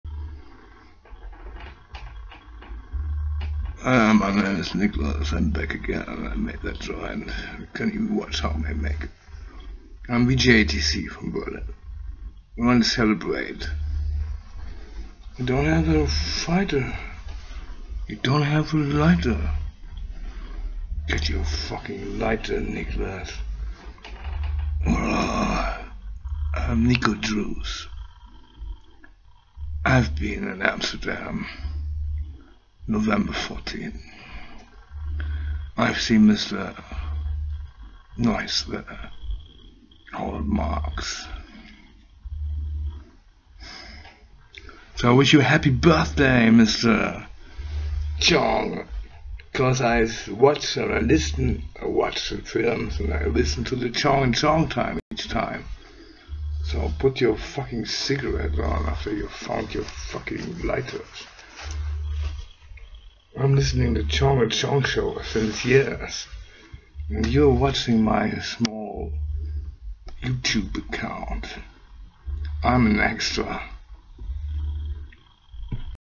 Herzlichen Glückwunsch zumGeburtstag, Herr Tommy Chong!
Zuckshows VJ ETC celebrates Mr. Chongs B - Day in bigB, Berlin.